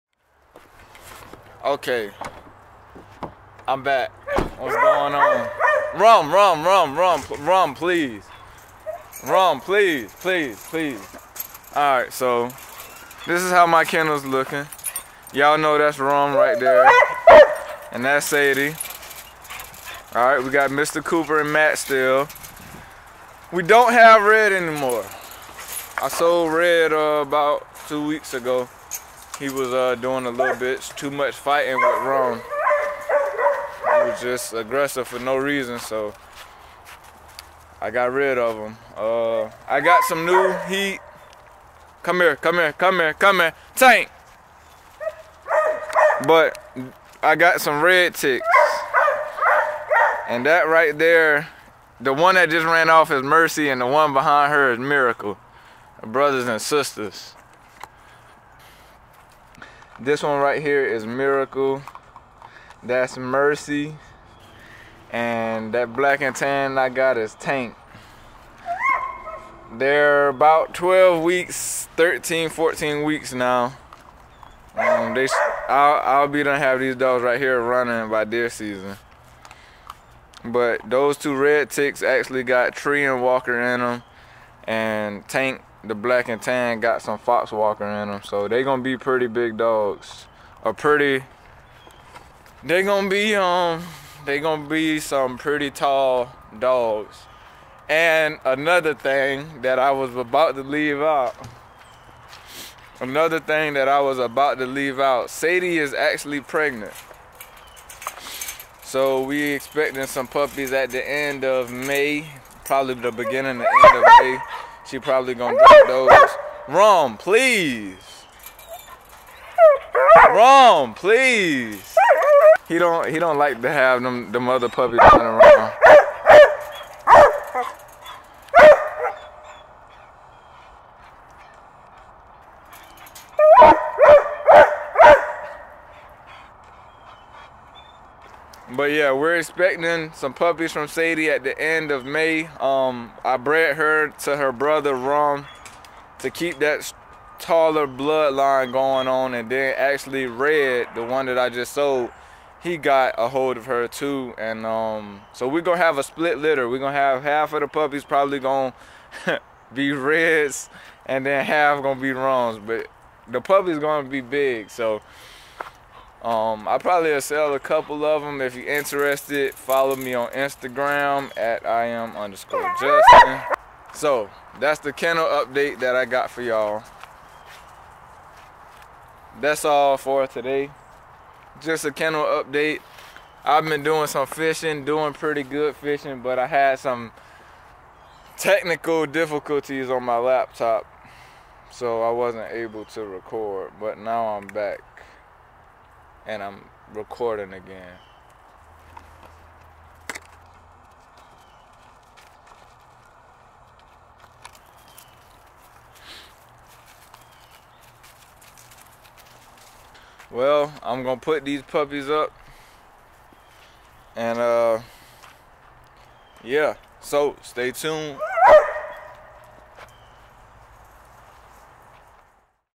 2022 Deer Dog Season Kennel Update!!
Im just giving you guys the run down on my new puppies for the upcoming season! More to Come! Sadie should be pregnant we are EXPECTING! Not 100% sure yet.